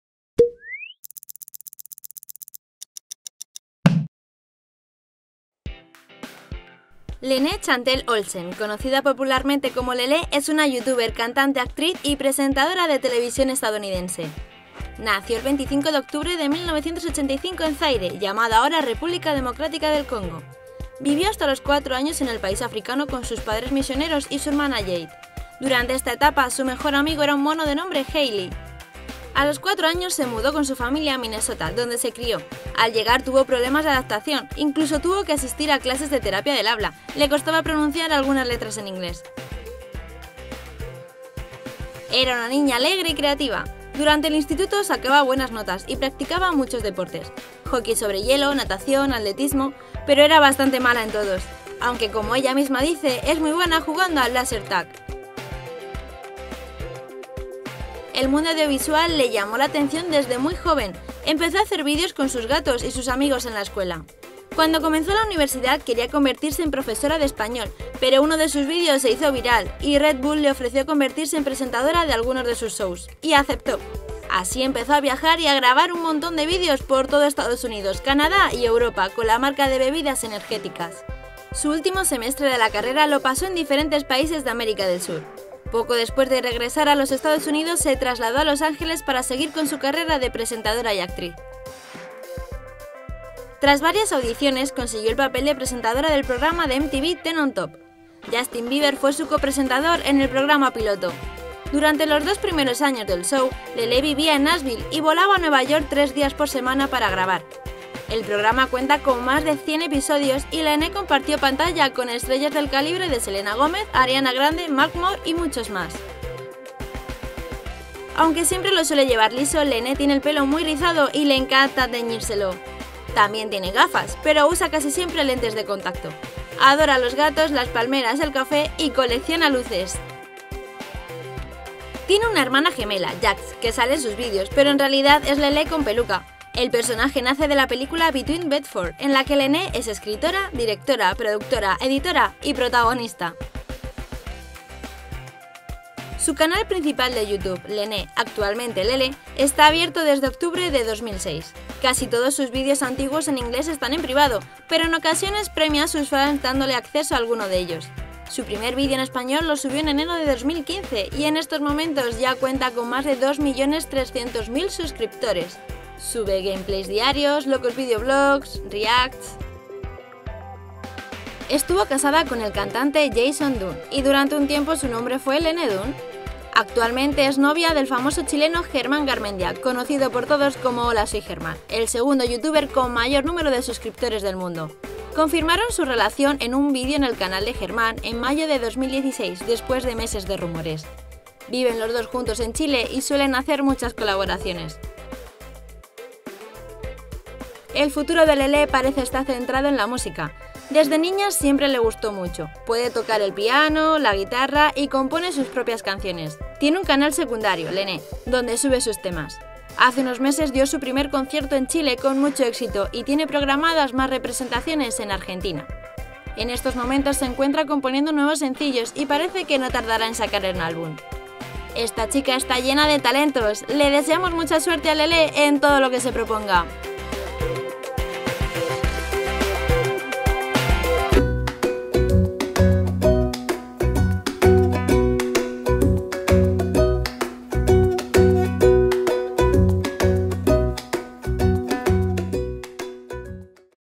LENAY (LELE) | Draw My Life
Este es el Draw My Life de Lele o Lenay, la youtuber con base en Chile y novia del conocido Germán Garmendia!!